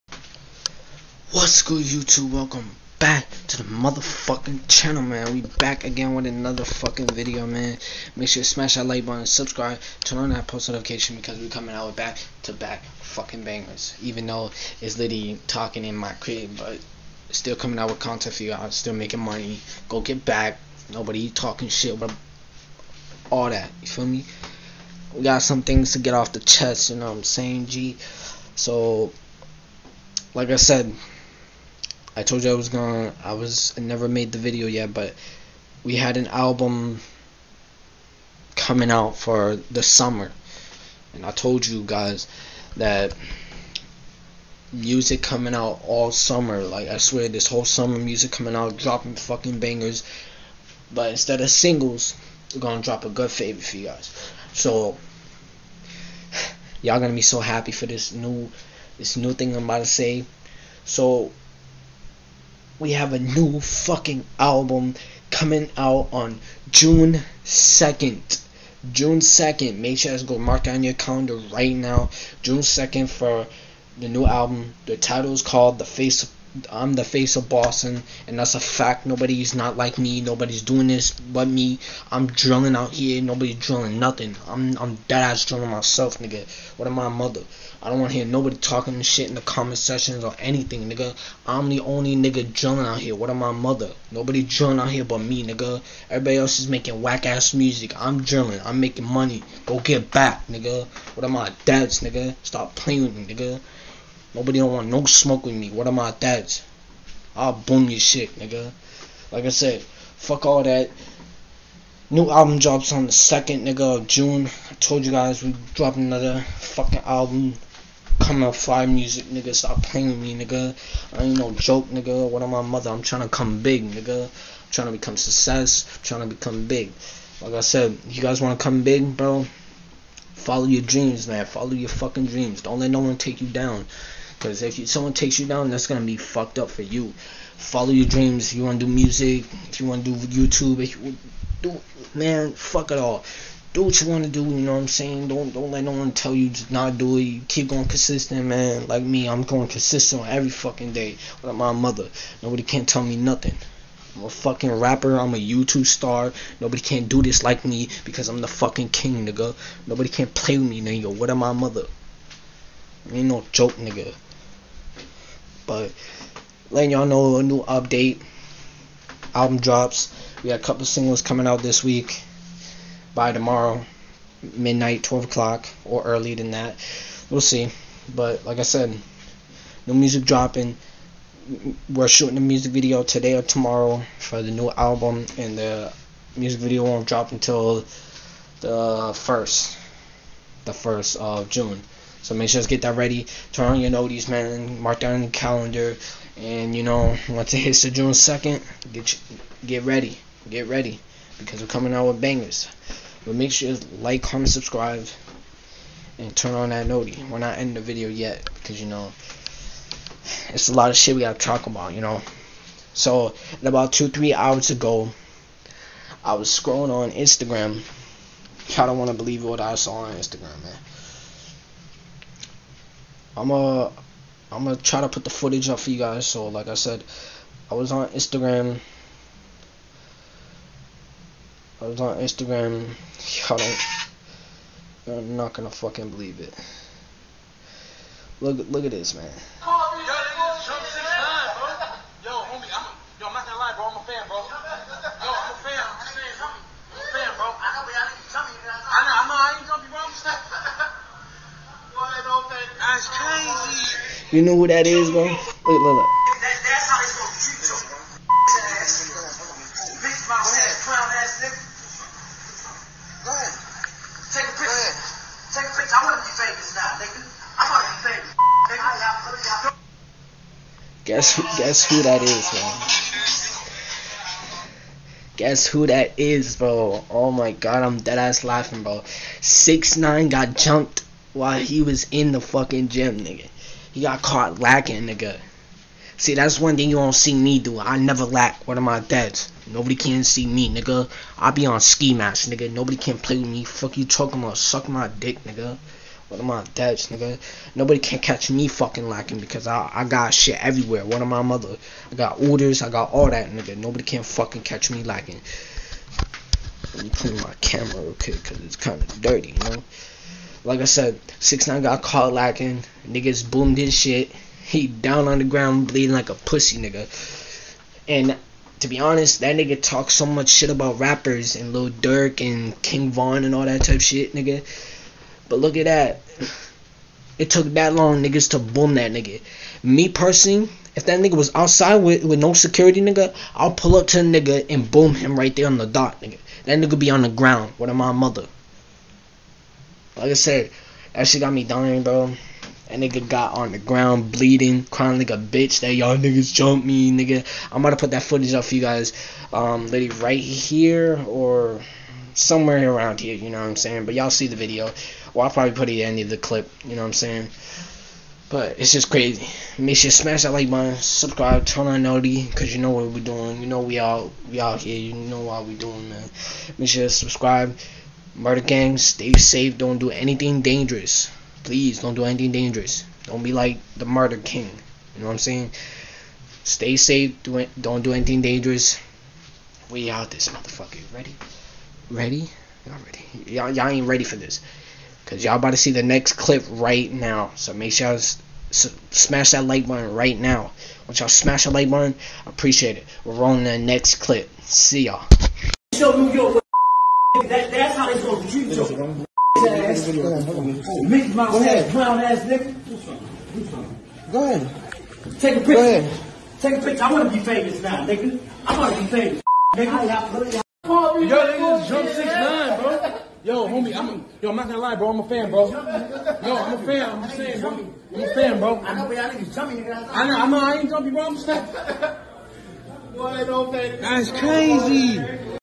New Album On June 2th | New Vlog | Talking About 6ix9ine Get Jump At the Gym | Full Video On My IG
Full Video On My IG